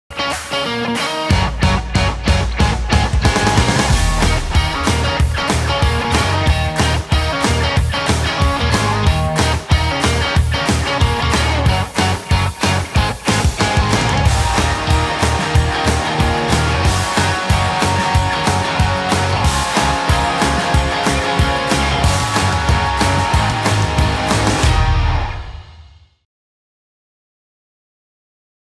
Hershey Road Trip Teaser
Tomorrow we're heading to America's Largest RV Show in Hershey, PA.  We're excited to be launching Camping.Tools at this iconic event.